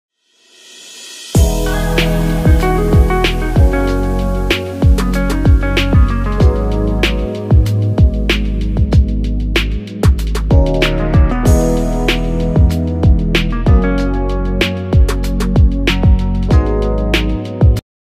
Upgrade Your Outdoor Space with Our Stylish Rattan Corner Sofa Set!"
Looking for the perfect addition to your patio or garden? Check out CHERISHOUTDOOR’s new wide rattan weaving corner sofa set! With its elegant aluminum alloy frame, unique handwoven design, and flexible configurations, this sofa set transforms any outdoor space into a stylish and cozy retreat. Don’t miss the oval-shaped coffee tables that complete the look! Ready to elevate your outdoor living? Watch now!